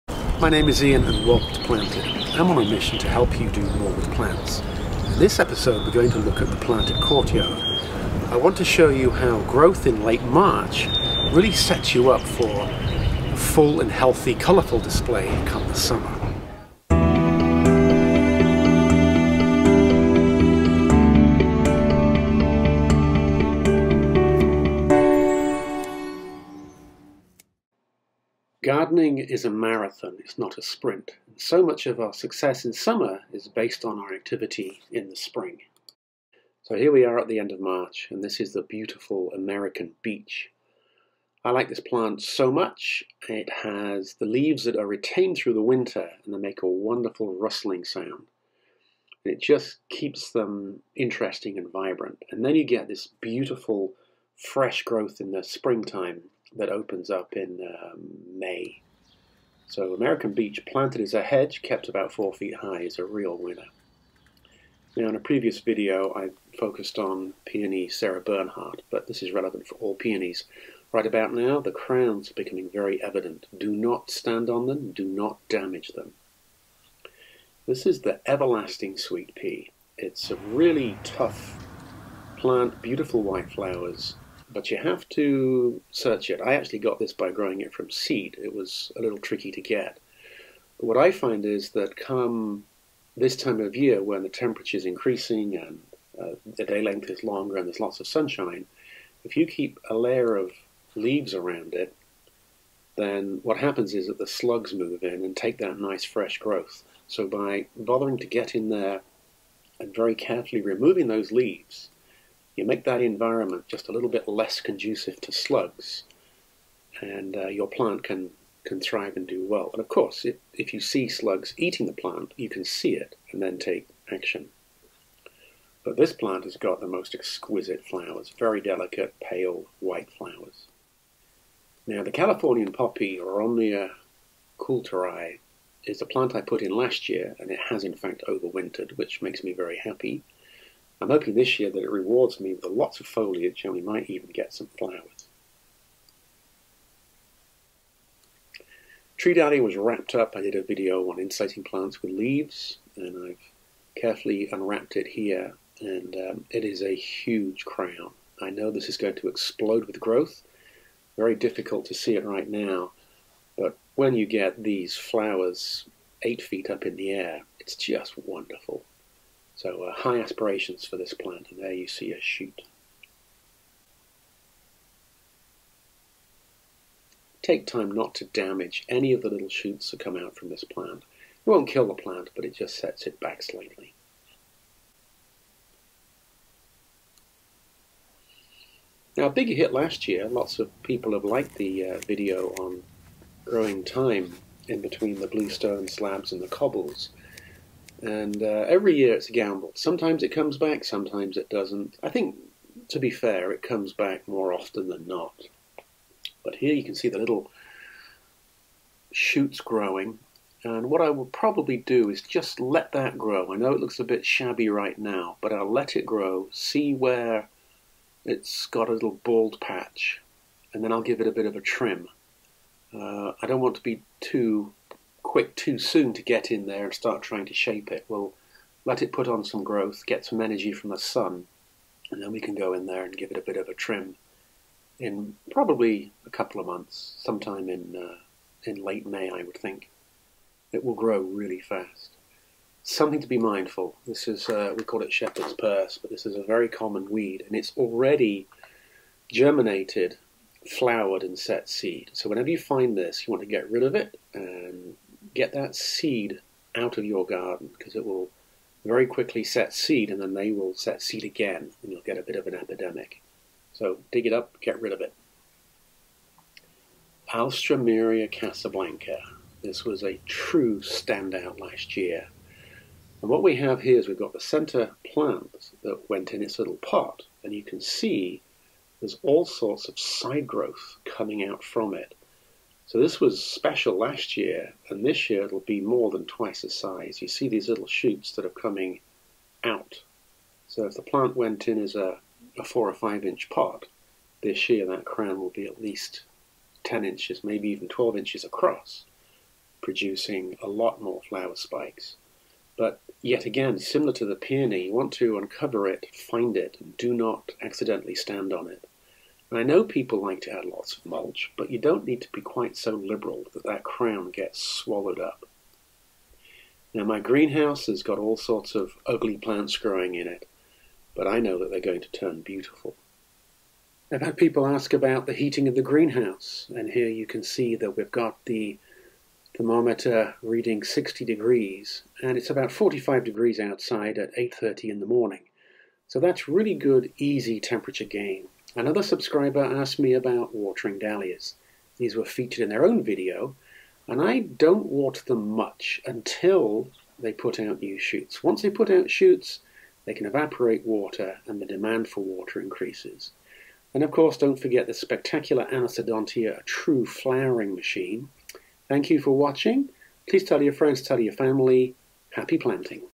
Plants emerge from their winter slumber - Late March
March is a magic month in Coastal New England, the clocks have changed the days are longer and warmer and plants are beginning to wake up from their winter slumber. Much of the glory of summer is connected to observing plant growth in March. In this video I show you some of my cherished plants in The Planted Courtyard and how they are growing. Get out not your garden and see what came through the winter - you might be surprised!